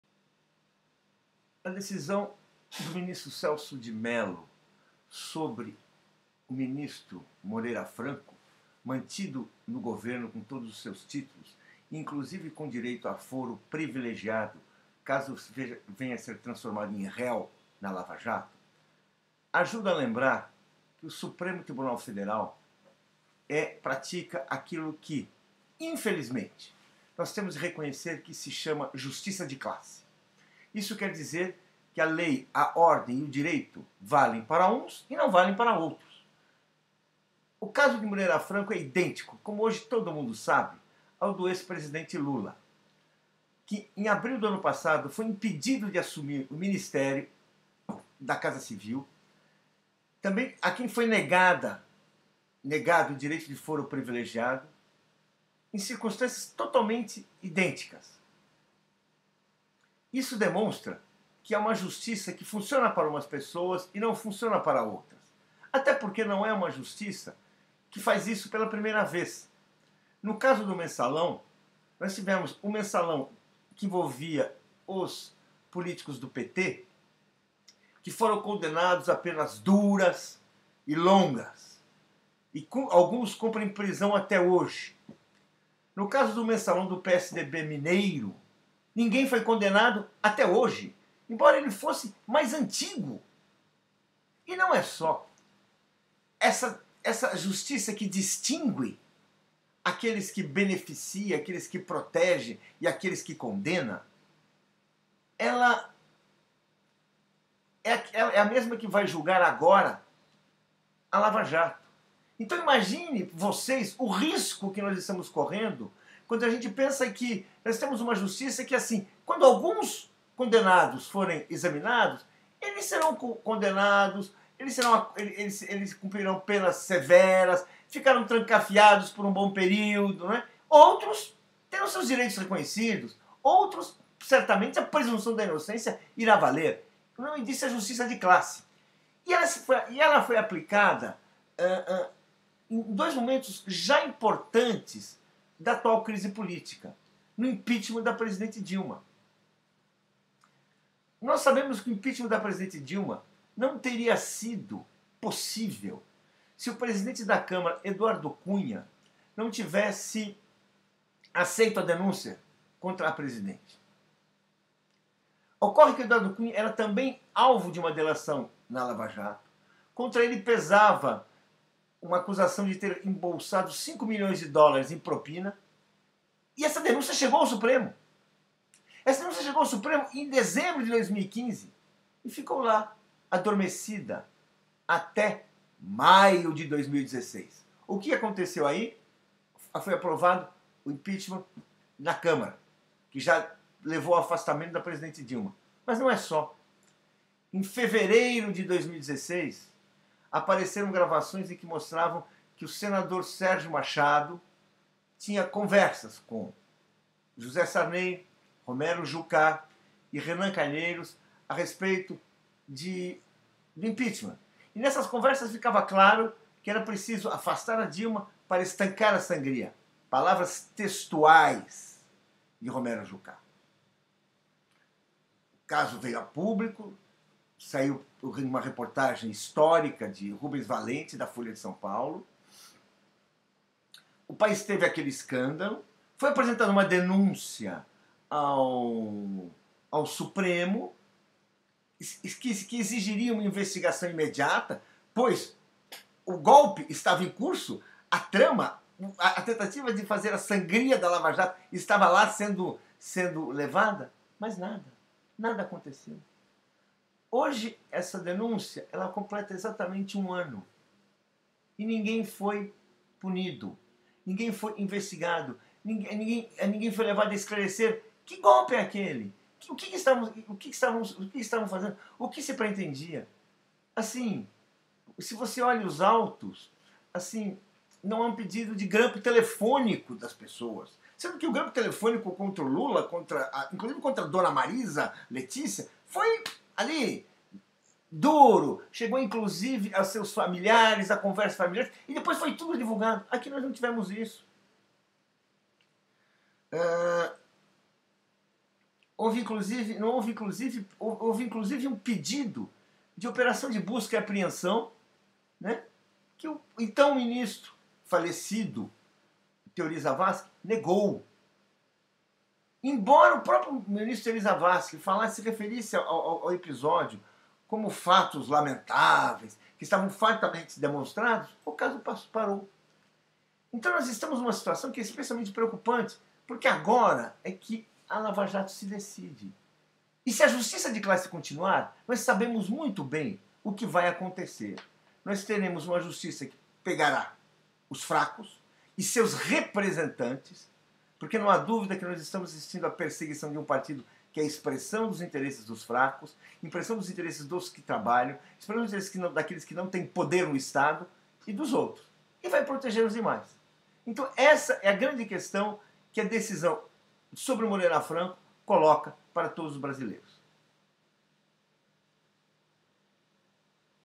Justiça de classe
Paulo Moreira Leite diz que agora o STF oficializou dois pesos e duas medidas